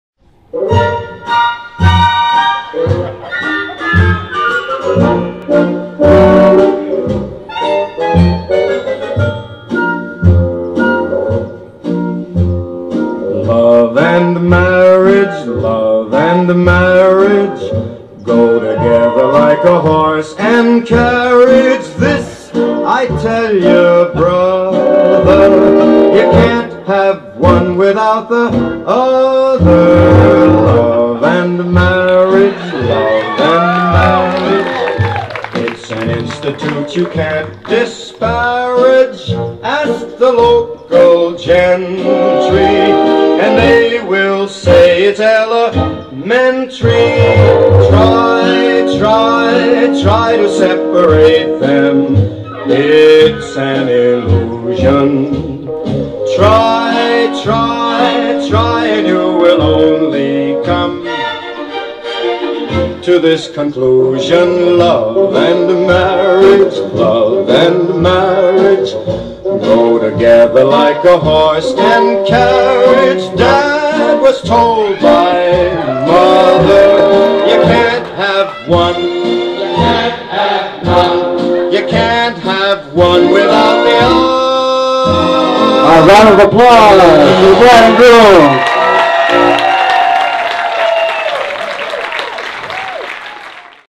Our first dance
John and Christine have their first dance at their wedding.